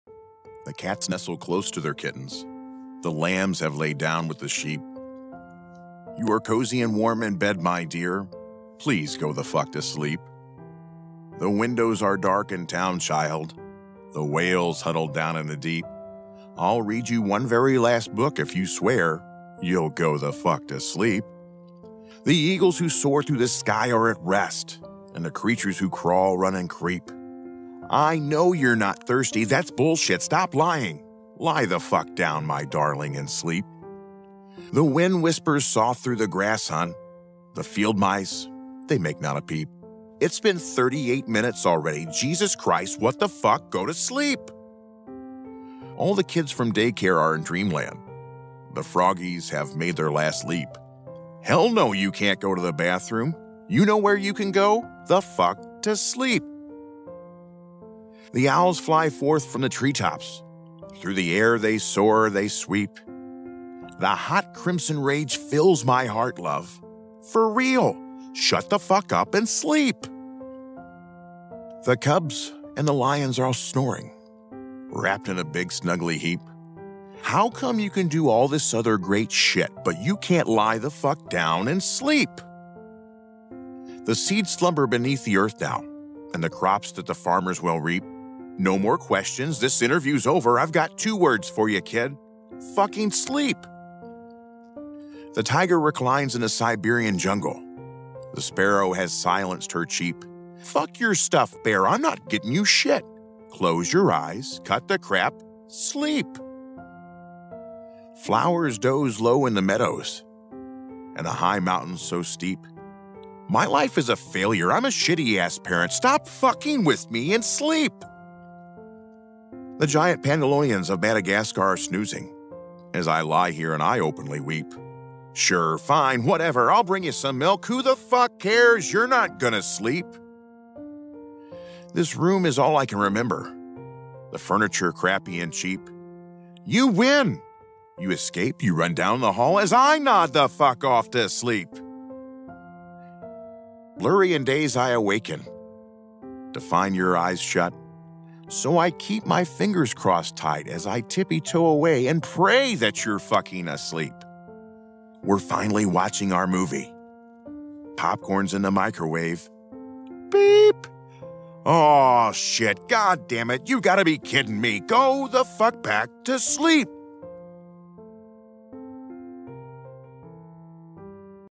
Lucas Chitwood Reads Go The Fuck To Sleep by Adam Mansbach
Lucas Chitwood reads the classic bedtime story, written by Adam Mansbach and illustrated by Ricardo Cortés.

This is for demonstration purposes only not for profit.